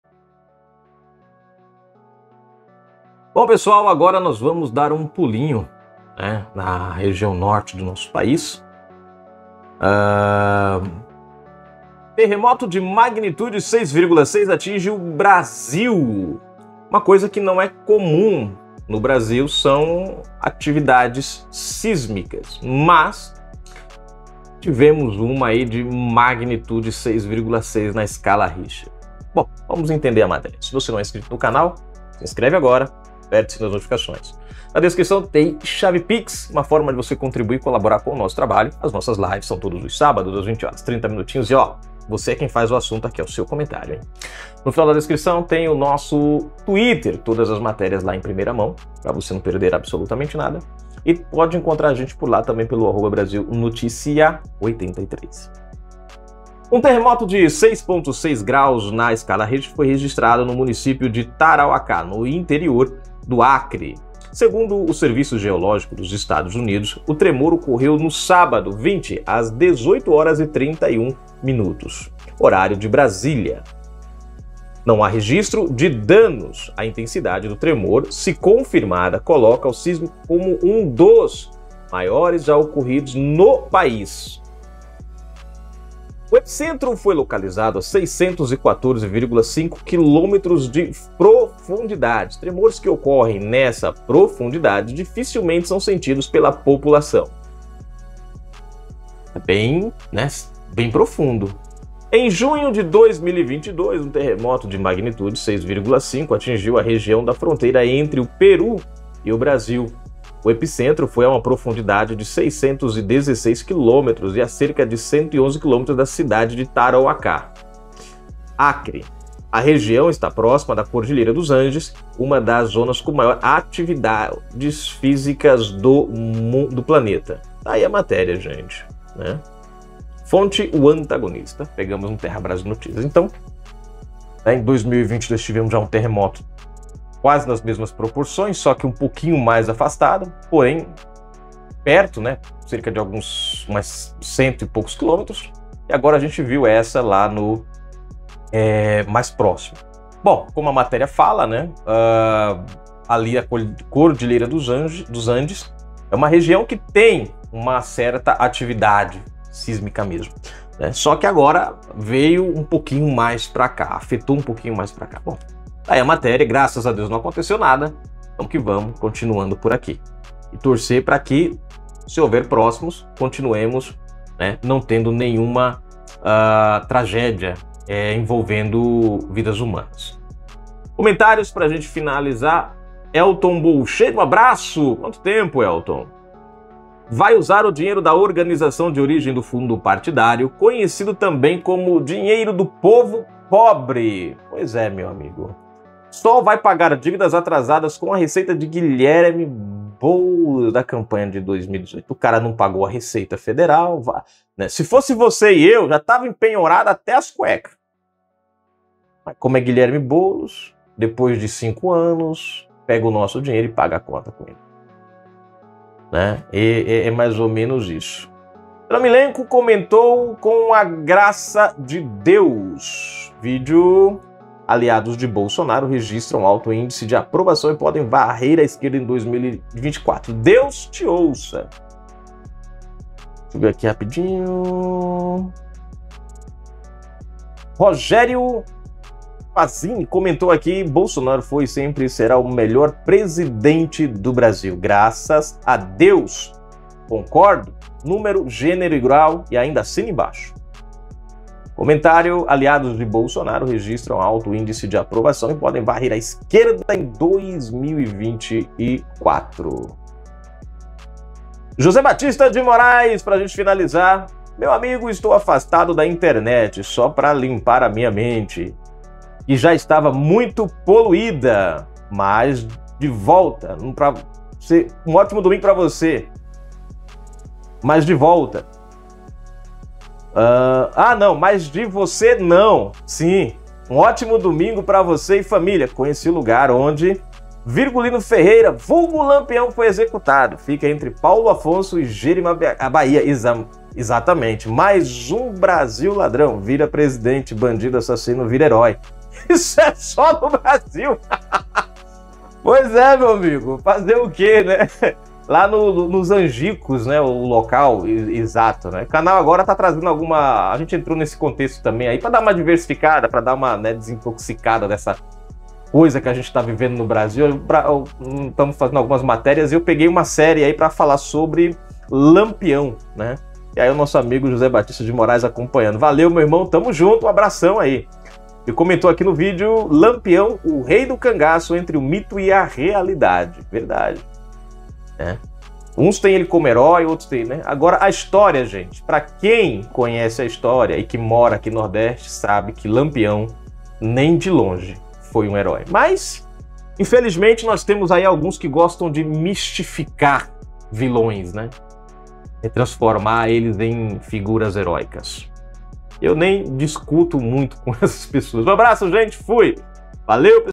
Terremoto de magnetude 6,6 é sentida no Acre!
Um terremoto de 6.6 graus na escala Richter foi registrado no município de Tarauacá, no interior do Acre. Segundo o Serviço Geológico dos Estados Unidos, o tremor ocorreu no sábado, 20, às 18h31 no horário de Brasília.
Não há registro de danos. A intensidade do tremor, se confirmada, coloca o sismo como um dos maiores já ocorridos no país.